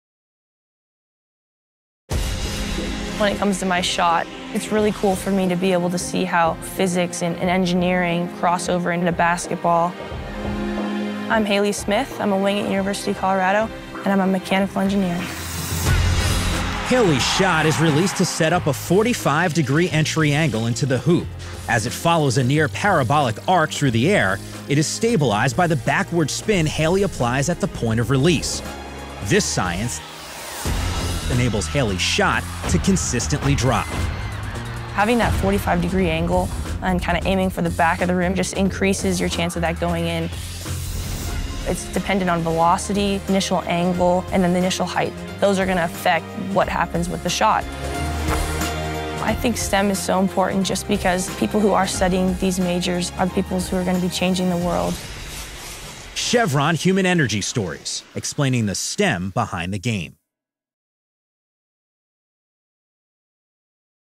Chevron HumanEnergyStories: Haley Smith
(Originally published on 02/09/2016) How do physics and engineering help Haley Smith, a mechanical engineering major, drain threes on the basketball court? Watch to see the science behind Haley's silky smooth shot.